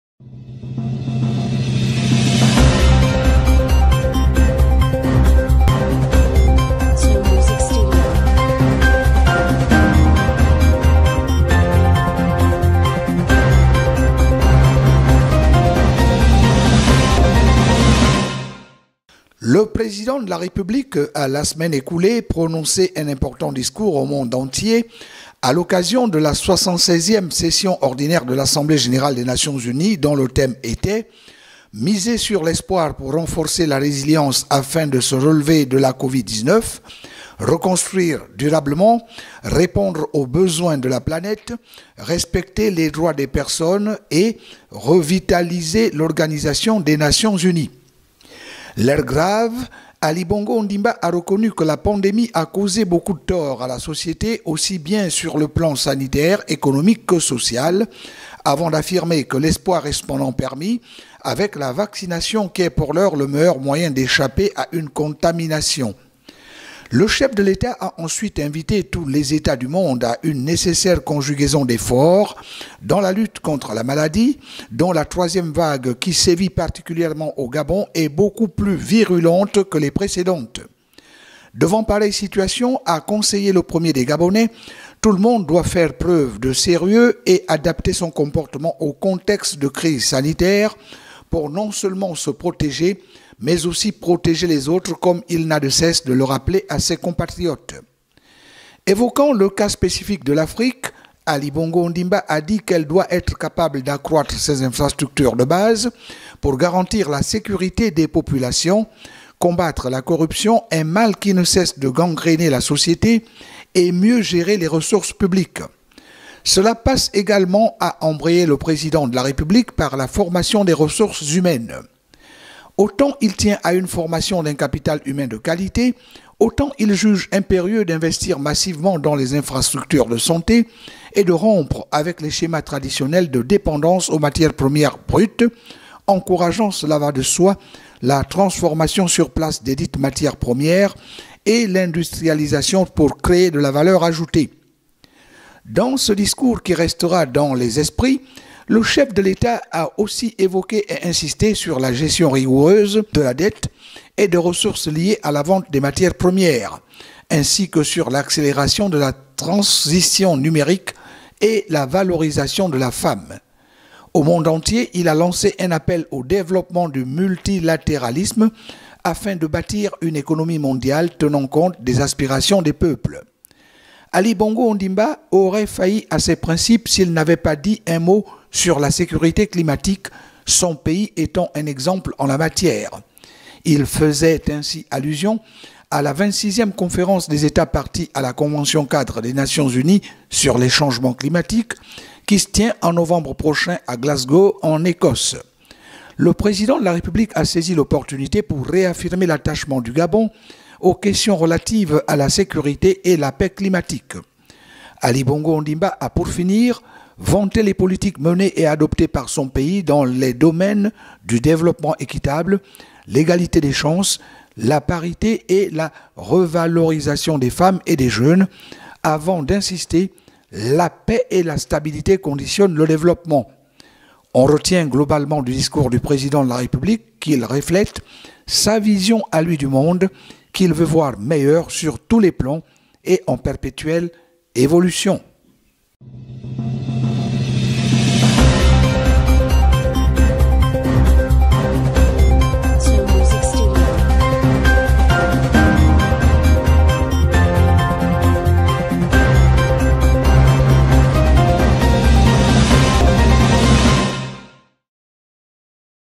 ACTUALITÉ PRÉSIDENTIELLE EN FRANÇAIS FACILE
Le président de la République a prononcé un important discours au monde entier, à l’occasion de la 76ème session ordinaire de l’Assemblée Générale des Nations-Unies dont le thème était : « Miser sur l’espoir pour renforcer la résilience afin de se relever de la Covid-19, reconstruire durablement, répondre aux besoins de la planète, respecter les droits des personnes et revitaliser l’Organisation des Nations-Unies ».